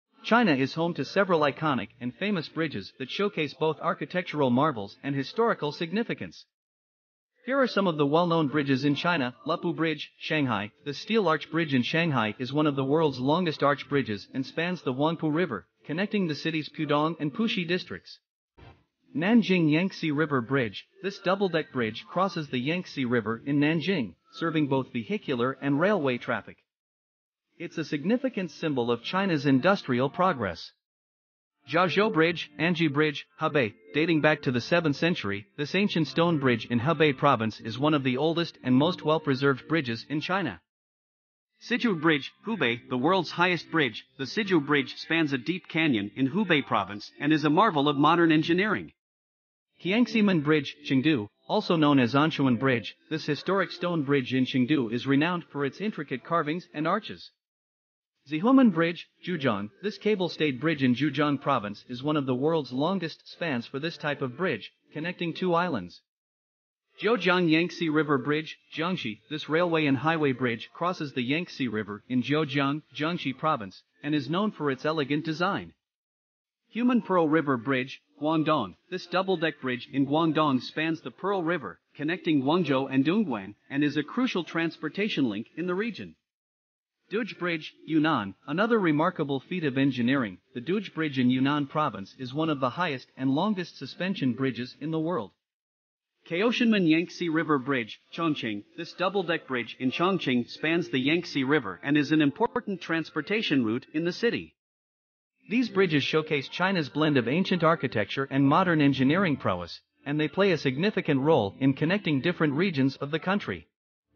China's Famous Bridges Architectural Marvels and Engineering Feats
China is home to several iconic and famous bridges that showcase both architectural marvels and historical significance. Here are some of the well-known bridges in China:

1) Lupu Bridge, Shanghai: This steel arch bridge in Shanghai is one of the world's longest arch bridges and spans the Huangpu River, connecting the city's Pudong and Puxi districts.

2) Nanjing Yangtze River Bridge: This double-deck bridge crosses the Yangtze River in Nanjing, serving both vehicular and railway traffic. It's a significant symbol of China's industrial progress.

3) Zhaozhou Bridge (Anji Bridge), Hebei: Dating back to the 7th century, this ancient stone bridge in Hebei Province is one of the oldest and most well-preserved bridges in China.

4) Siduhe Bridge, Hubei: The world's highest bridge, the Siduhe Bridge spans a deep canyon in Hubei Province and is a marvel of modern engineering.

5) Qianximen Bridge, Chengdu: Also known as Anshun Bridge, this historic stone bridge in Chengdu is renowned for its intricate carvings and arches.

6) Xihoumen Bridge, Zhejiang: This cable-stayed bridge in Zhejiang Province is one of the world's longest spans for this type of bridge, connecting two islands.

7) Jiujiang Yangtze River Bridge, Jiangxi: This railway and highway bridge crosses the Yangtze River in Jiujiang, Jiangxi Province, and is known for its elegant design.

8) Humen Pearl River Bridge, Guangdong: This double-deck bridge in Guangdong spans the Pearl River, connecting Guangzhou and Dongguan, and is a crucial transportation link in the region.

9) Duge Bridge, Yunnan: Another remarkable feat of engineering, the Duge Bridge in Yunnan Province is one of the highest and longest suspension bridges in the world.

10) Chaotianmen Yangtze River Bridge, Chongqing: This double-deck bridge in Chongqing spans the Yangtze River and is an important transportation route in the city.

These bridges showcase China's blend of ancient architecture and modern engineering prowess, and they play a significant role in connecting different regions of the country.